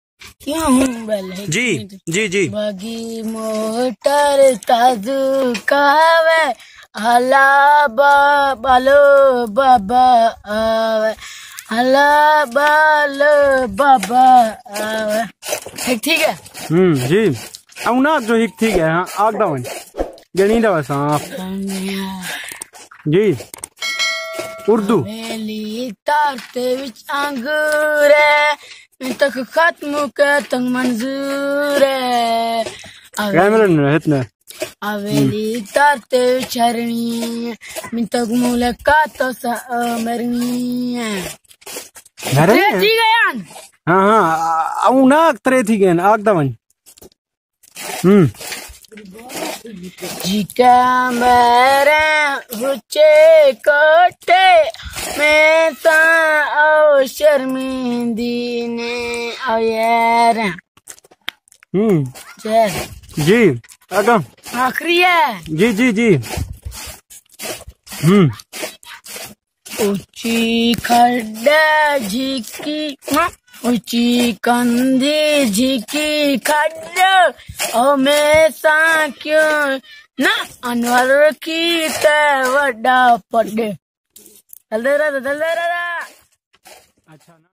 Chota Fankar | Little Singer | little village boy singing | Koh Sulaiman TV
In this video you will enjoy the singing of Kamran jaffar a little village singer